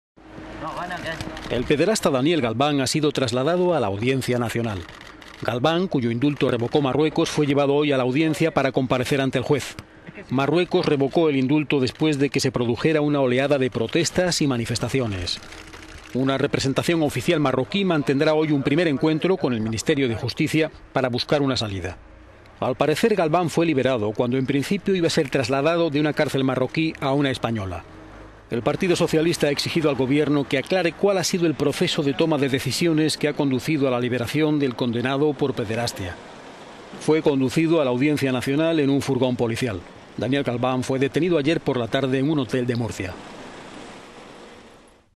El pederasta Daniel Galván ha sido trasladado a la Audiencia Nacional
Galván cuyo indultó revocó Marruecos, fue llevado hoy a la Audiencia Nacional para comparecer...

Galván cuyo indultó revocó Marruecos, fue llevado hoy a la Audiencia Nacional para comparecer ante el juez. Marruecos revocó el indulto después de que se produjera una oleada de protestas y manifestaciones.

Una representación oficial marroquí mantendrá hoy un primer encuentro con el Ministerio de Justicia español para buscar una salida. Galván fue liberado cuando al parecer iba a ser trasladado de una cárcel marroquí a una española.